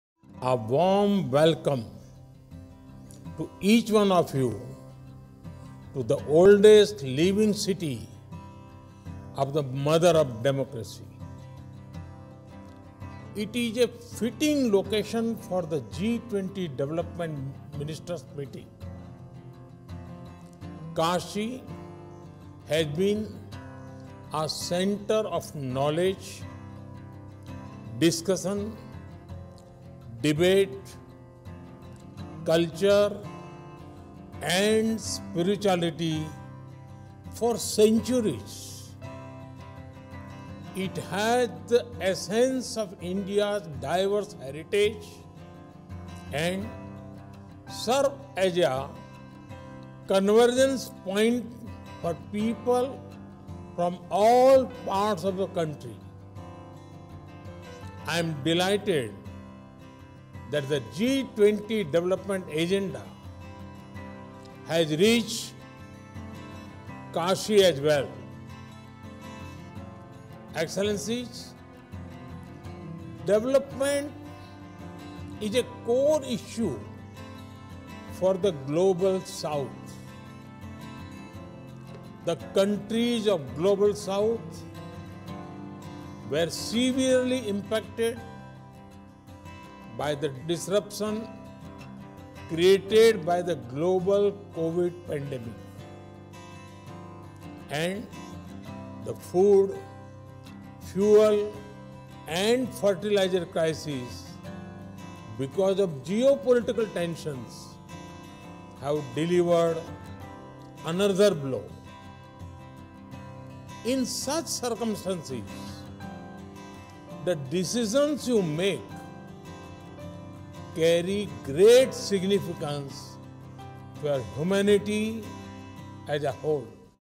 Unleashing Potential: Global South's Development Journey Amidst the CovidPandemic
Development is a core issue for GlobalSouth. The countries of the Global South were severely impacted by the disruption created by the global Covid pandemic:: PM @NarendraModi